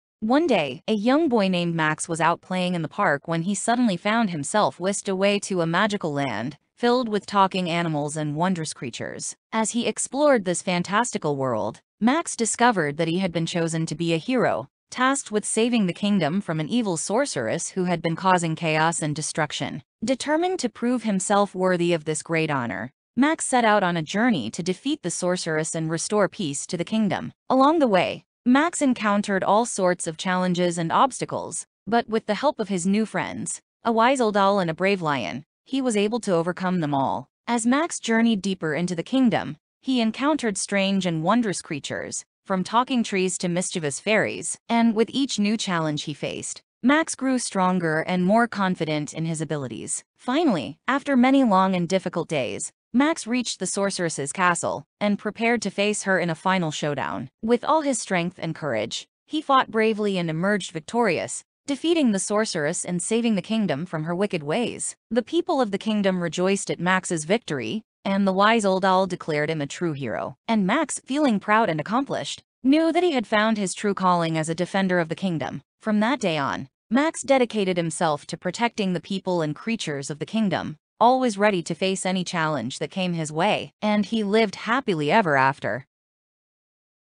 Max, The Protector
One day, a young boy named Max was out playing in the park when he suddenly found himself whisked away to a magical land filled with talking animals and wondrous creatures.